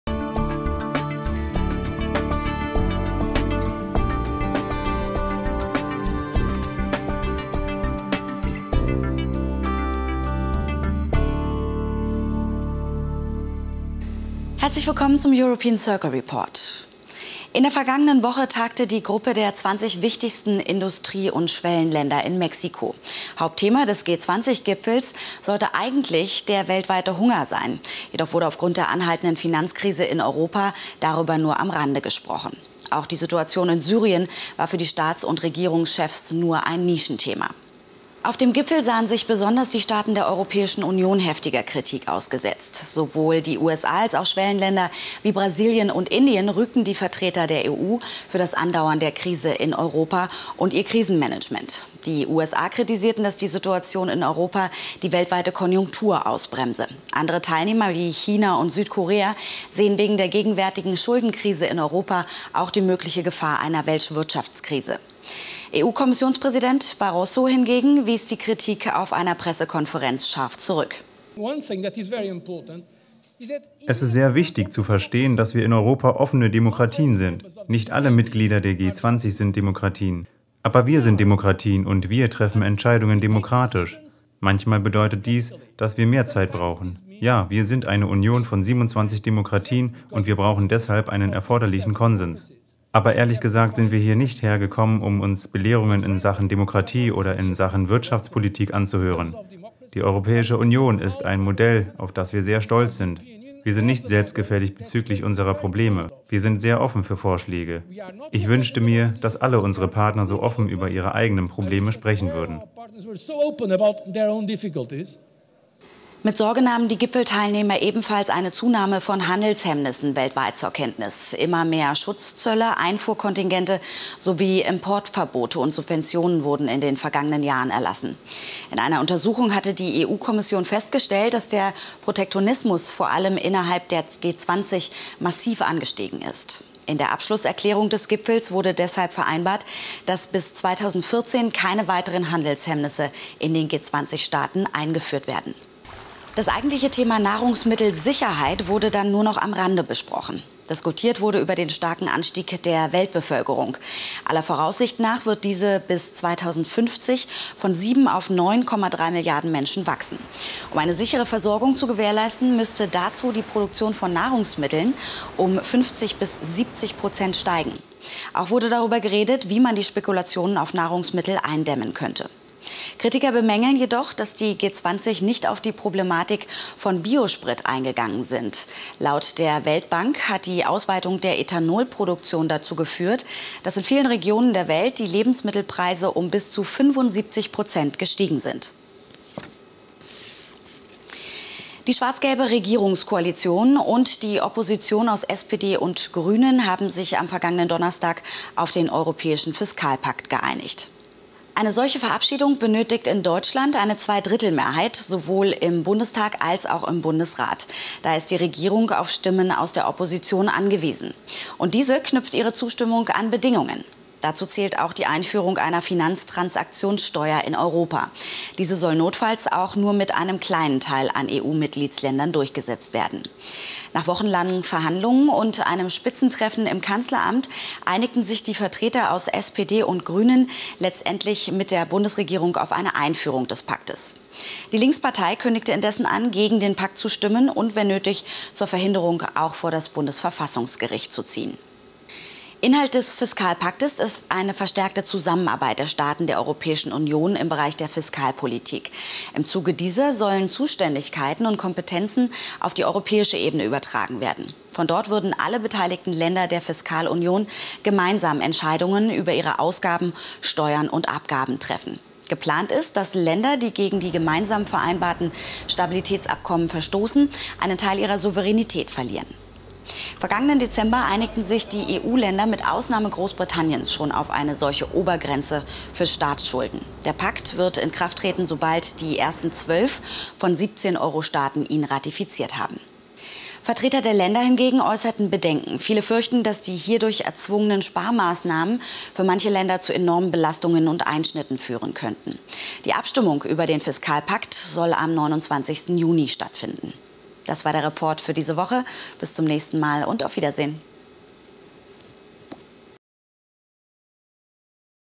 The European Circle - Report 209 - 25. Juni 2012 - G-20 Gipfel in Mexico
In der vergangenen Woche tagte die Gruppe der zwanzig wichtigsten Industrie- und Schwellenländer in Mexiko. Hauptthema des G20 Gipfels sollte eigentlich der weltweite Hunger sein, jedoch wurde aufgrund der anhaltenden Finanzkrise in Europa darüber nur am Rande gesprochen. Auch die Situation in Syrien war für die Staats und Regierungschefs nur ein Nischenthema.

Die schwarz-gelbe Regierungskoalition und die Opposition aus SPD und Grünen haben sich am vergangenen Donnerstag auf den europäischen Fiskalpakt geeinigt.